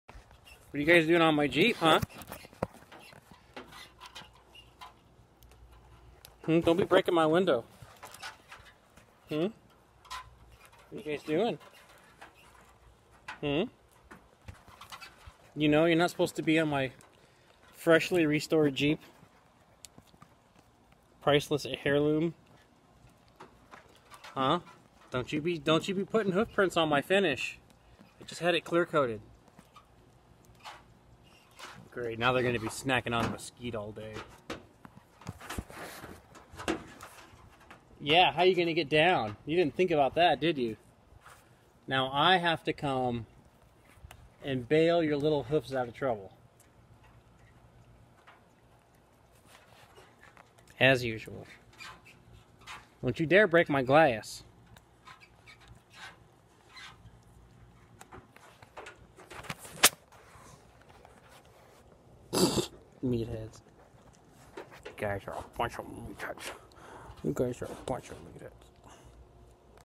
my pet goats love my new willys jeep
my pet goats think my jeep is a gymnasium built for them only